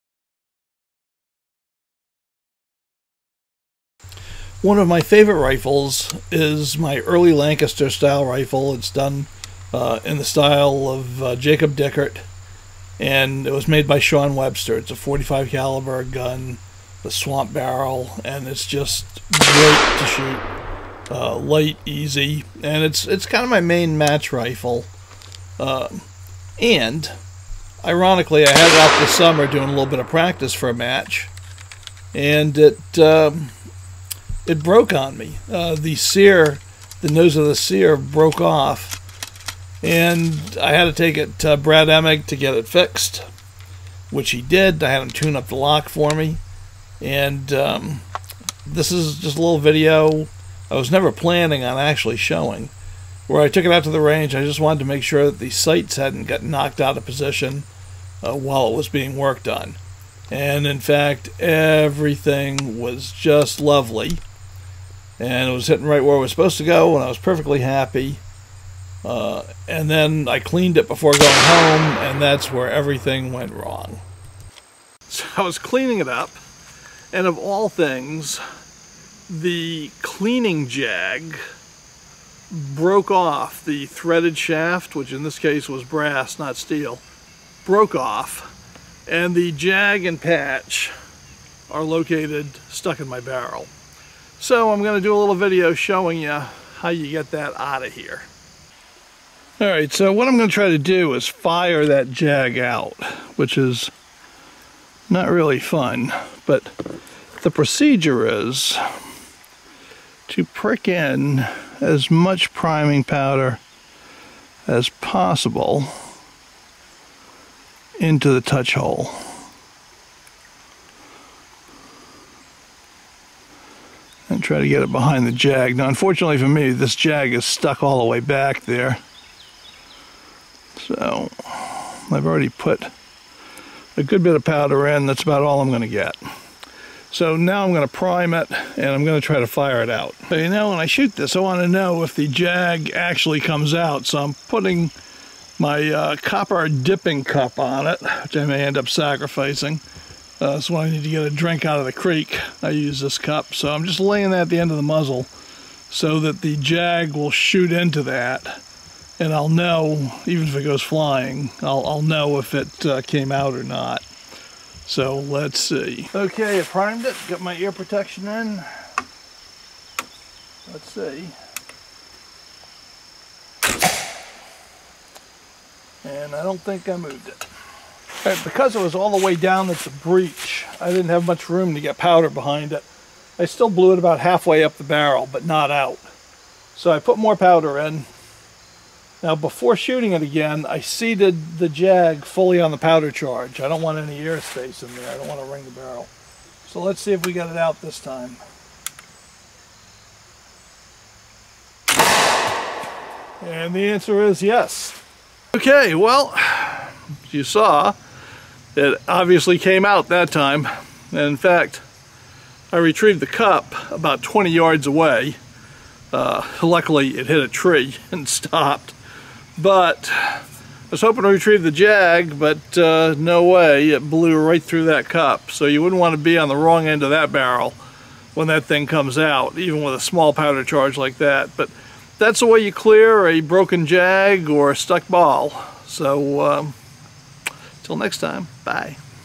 Clearing a stuck cleaning jag
In this video I’ll show you how to clear a broken cleaning jag out of the rifle’s bore when you are in the field.

This method will also work should you load a ball without a powder charge…also known as “dry-balling”